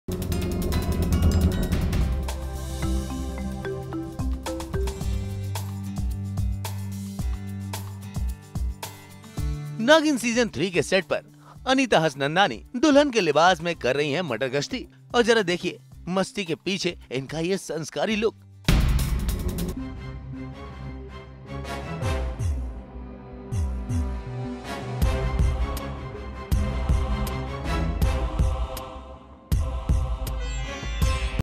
Naagin 3 -Anita Hassnandani in Lehenga
A behind the scene video of Naagin 3 with Anita